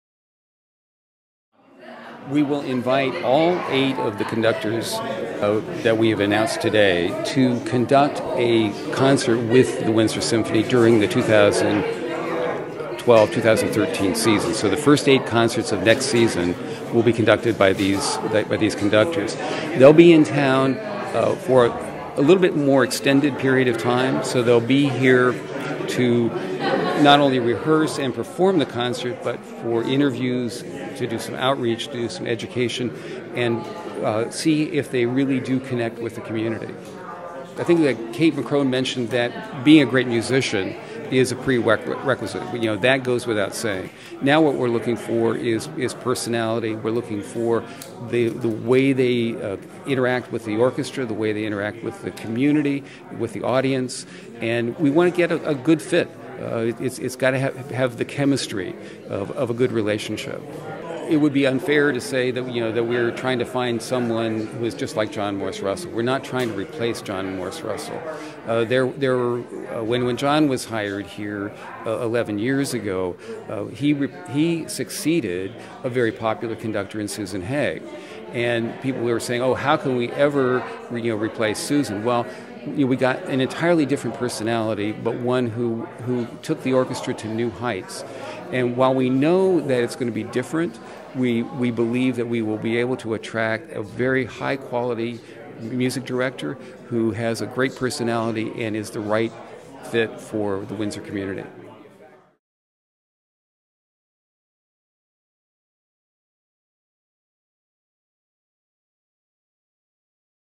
WSO Idol: The Search For John Morris Russell's Replacement
Jeth Mill, executive director of the Windsor Symphony Orchestra, talks about the search for a new music director for the WSO.